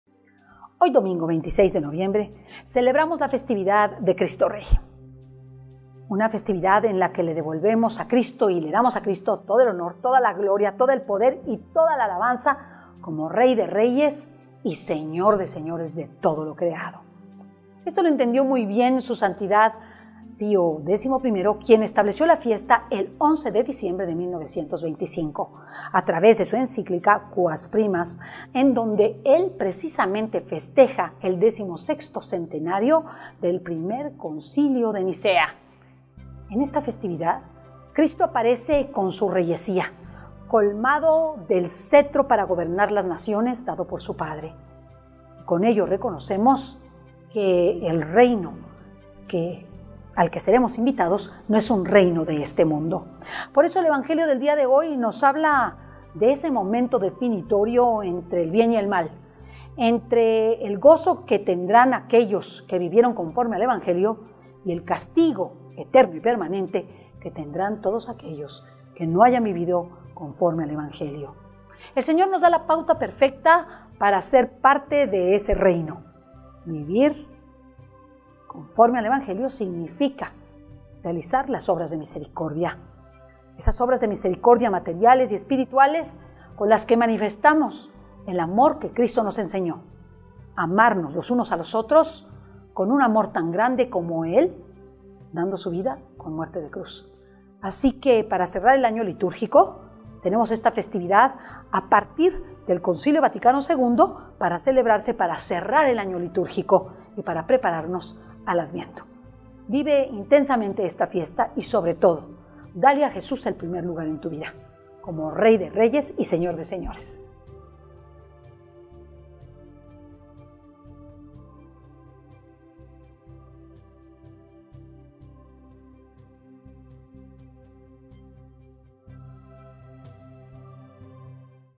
Reflexión del Evangelio Domingo 26 de Noviembre - Festividad de Cristo Rey
Festividad de Cristo Rey: entonces irán estos al castigo eterno y los justos a la vida eterna.